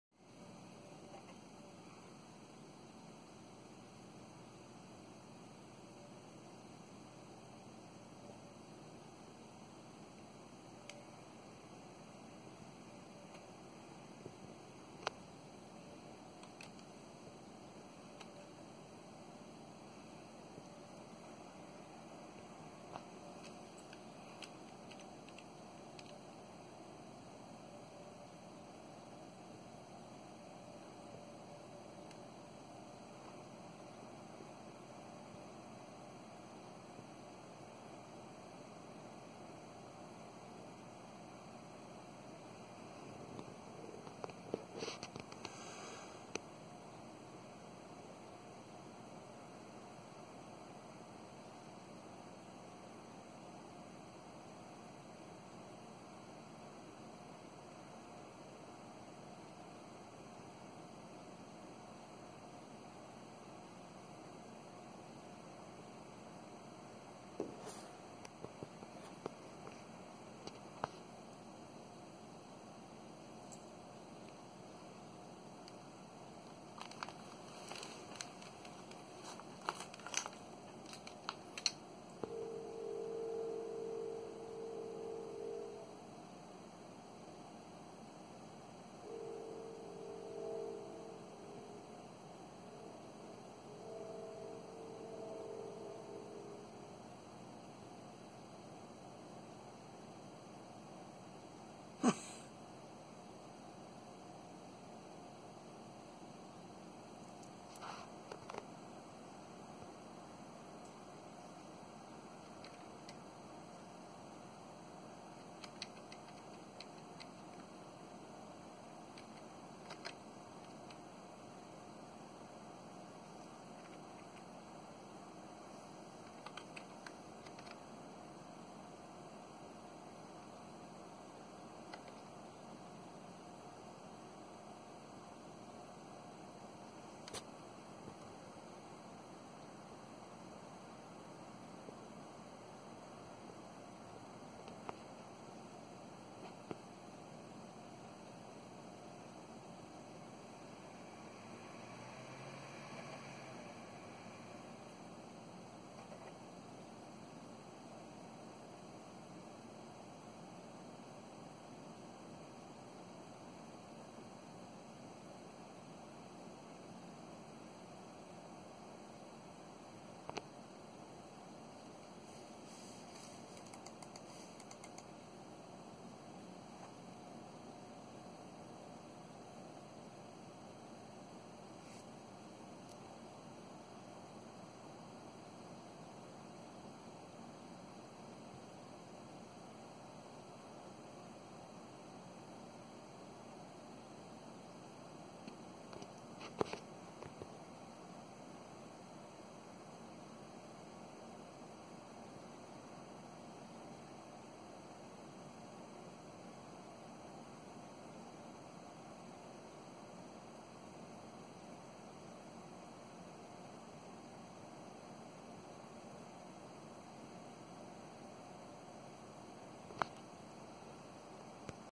New EVGA GTX 780 is died.
Just installed a Nvidia GTX 780 in my workstation replacing a GTX 460. Seems the new GPU is dead.